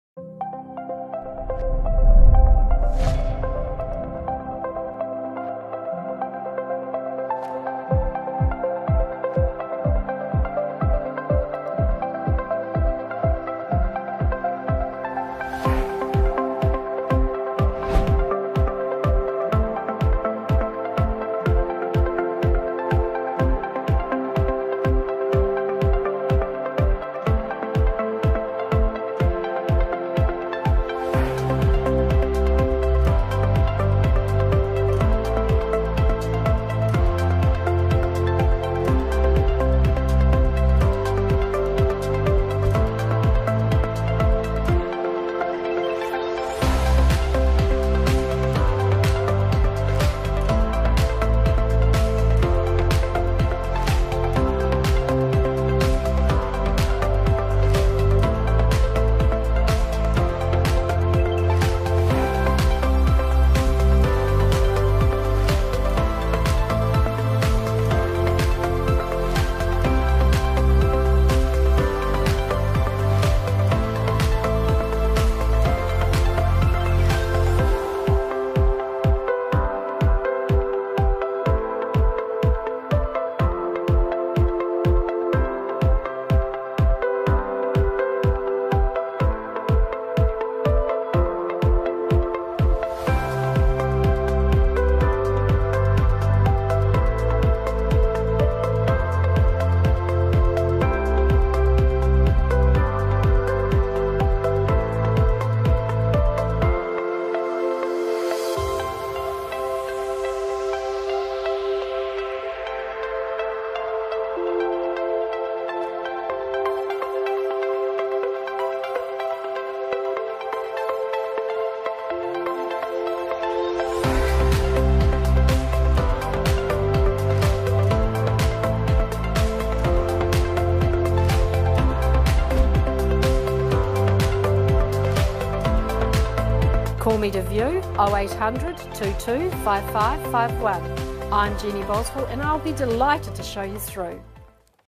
37 Chippendale Crescent, Palmerston North
37 Chippendale Crescent, Highbury
4 🛏️ 2 🛁 2 🚗

*Light and sunny low maintenance brick home with plenty of room for a family here!
*858m2 family sized section. Living flows out to a sunny patio, perfect for outdoor entertaining. Overlooks the reserve.
*Open plan kitchen/dining/family. Separate spacious lounge.
*Well-equipped kitchen with walk-in pantry and plenty of cupboards.
*Upgraded carpet - Cosy home with gas heating.
*Main bathroom shower, vanity and spa bath. Separate toilet.
*Four light-filled bedrooms boasting wardrobes. Three bedrooms open to the backyard.
*Master bedroom boasts an ensuite and walk-in wardrobe.
*Spa area - Separate laundry - Double garage.
*Close to Pioneer Highway Shopping Centre, New World and Warehouse.

BE QUICK to view!! Don't miss out! Call Jeannie on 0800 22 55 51 or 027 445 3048.